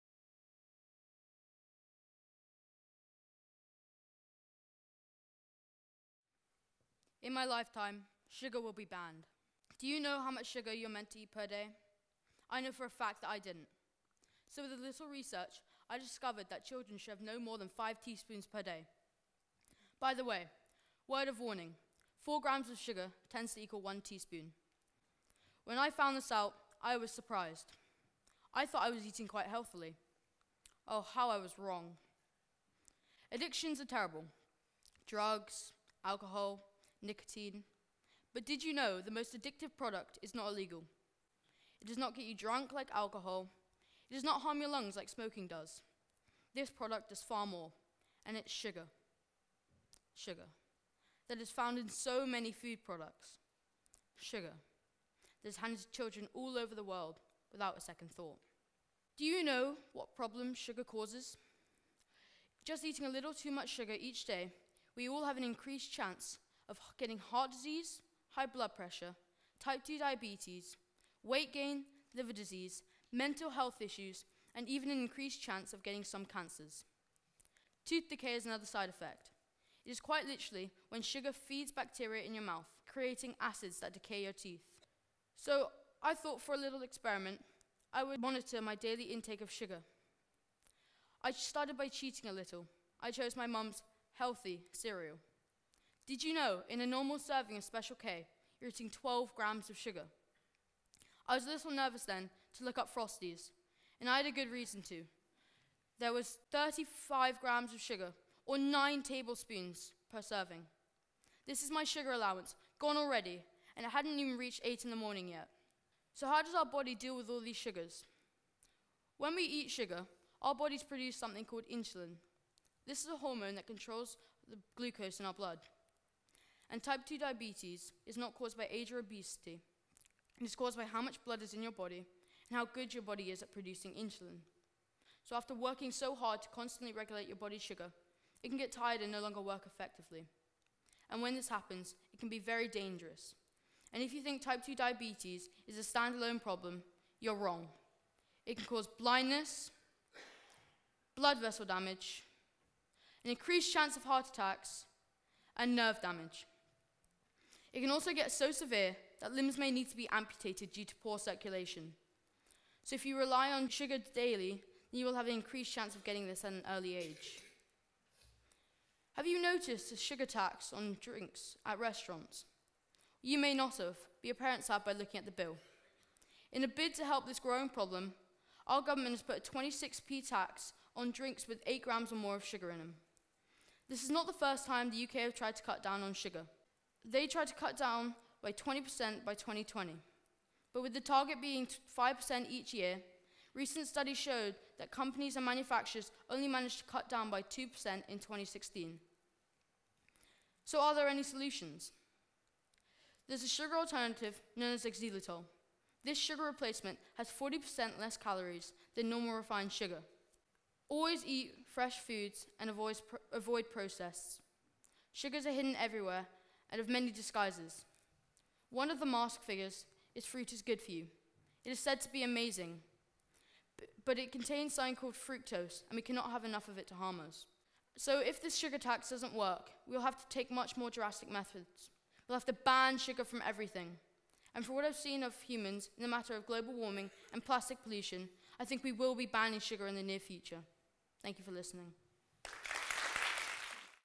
In my lifetime sugar will be banned | William Ransley | TEDxYouth@DPL
William speaks passionately about why we should be careful with sugar consumption in our diet and also how sugar is affecting our environment. In my lifetime sugar will be banned This talk was given at a TEDx event using the TED conference format but independently organized by a local community.